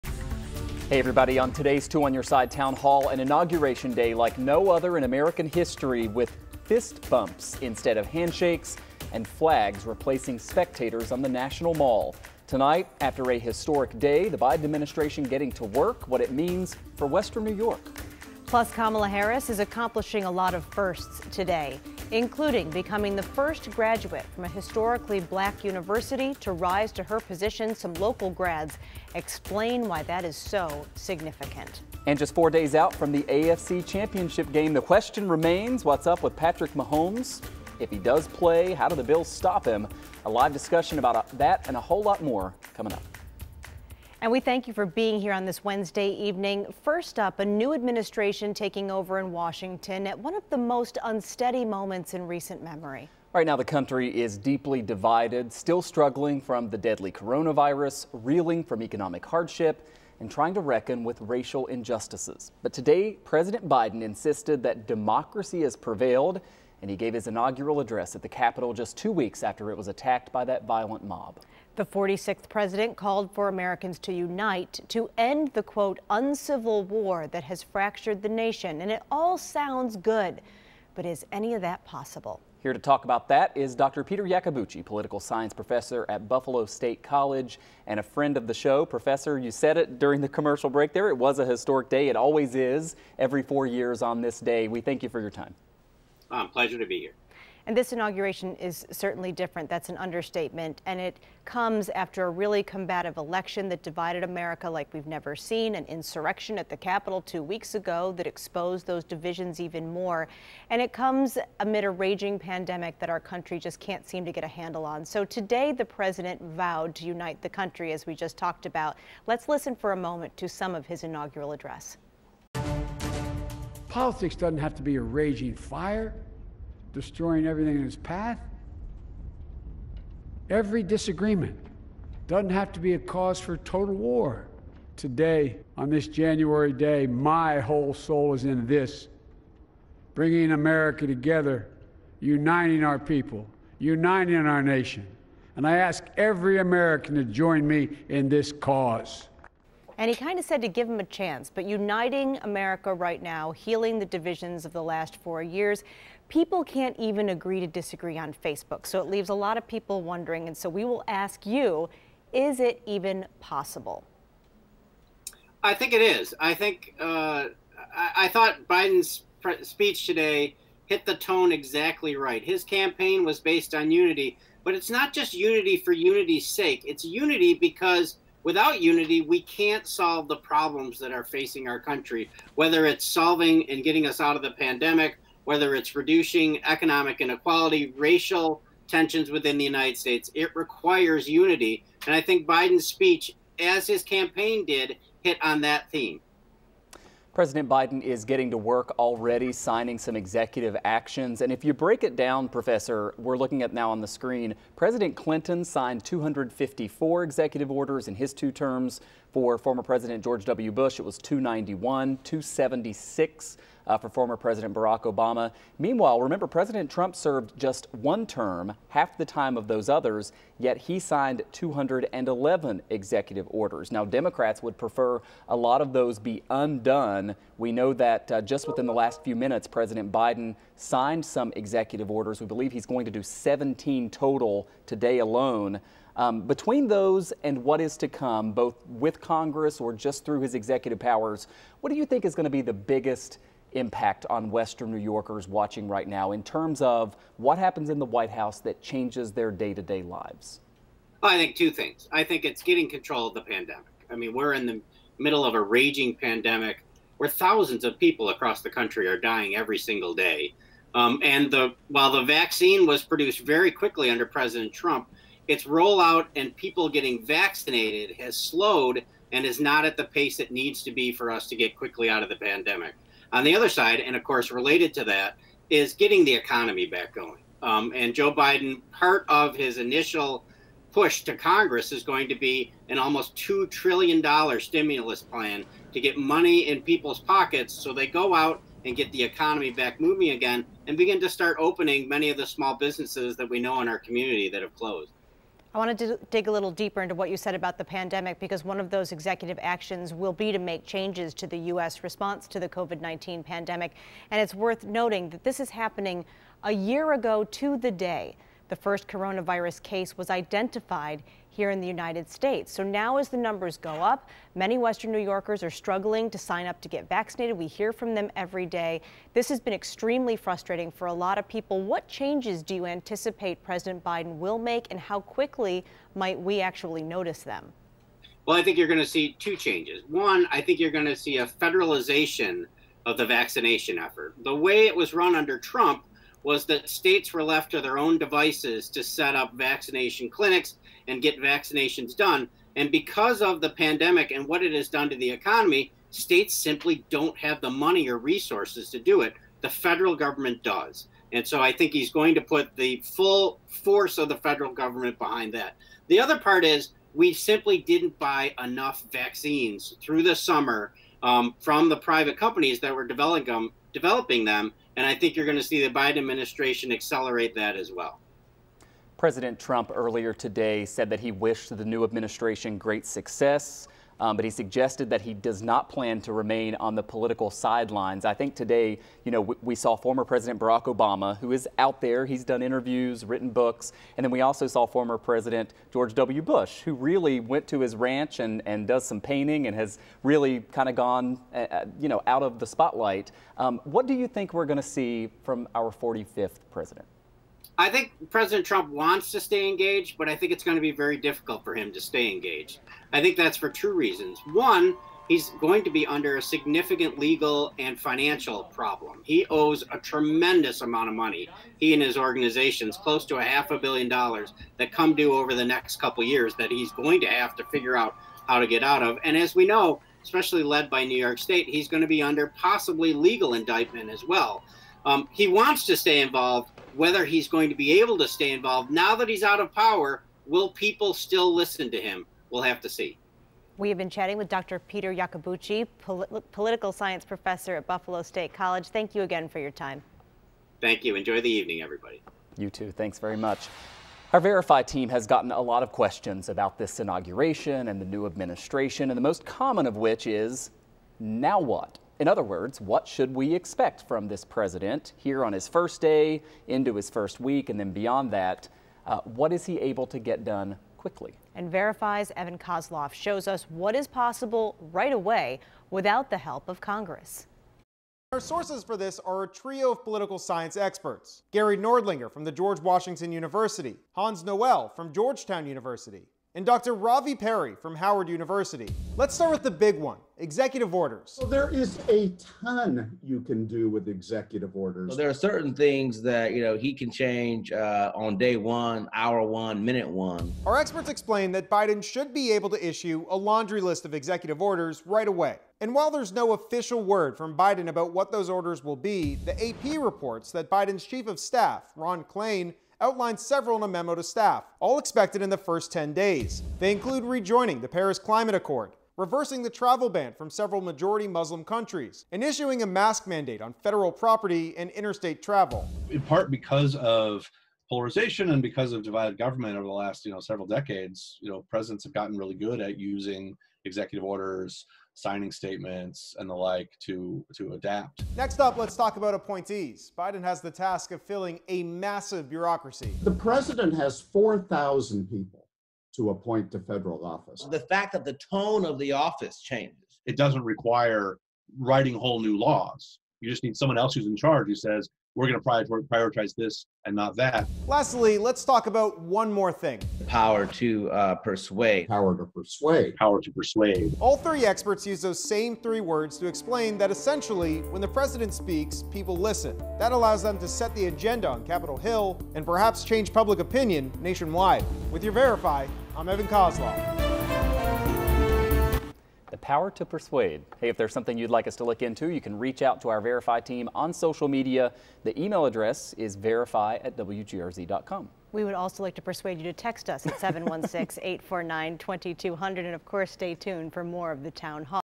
Wednesday Town Hall Segment One
Dr. Peter Yacobucci... political science professor at Buffalo State College talks about the inauguration